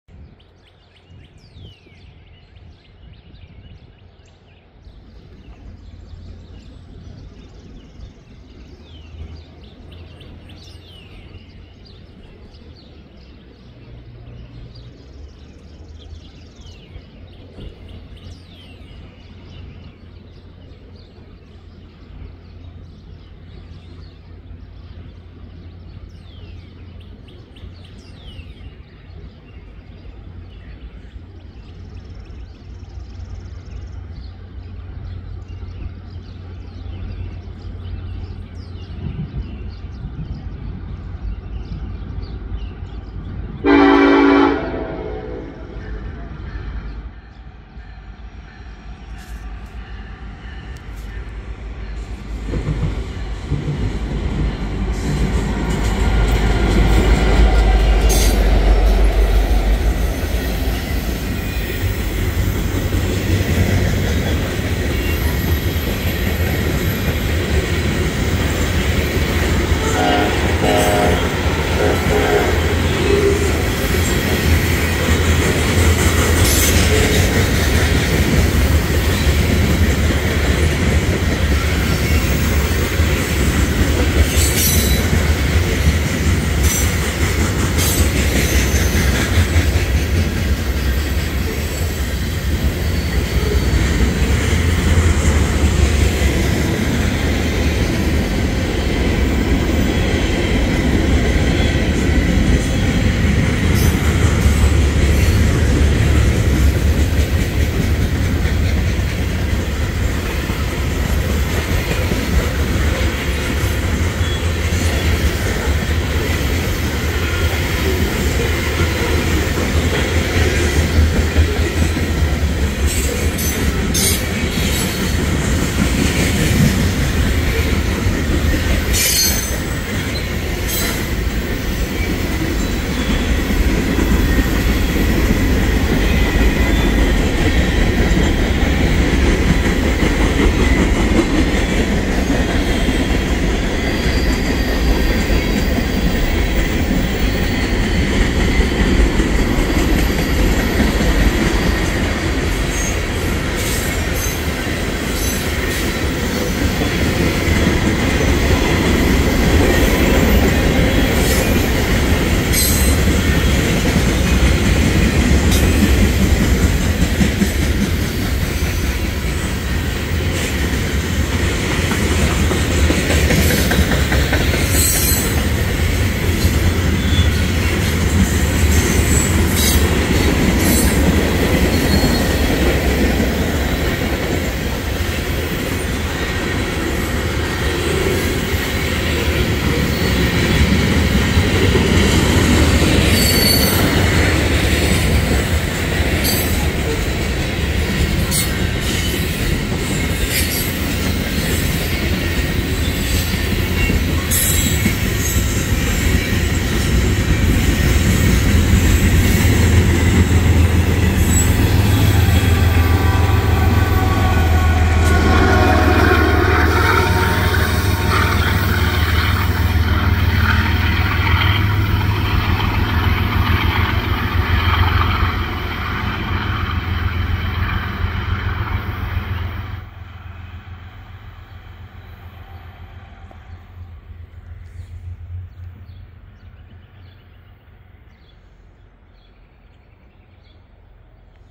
Norfolk Southern #1159 leads a 113 car manifest at Lewistown, PA (05/07/2024)
10K, on the shorter side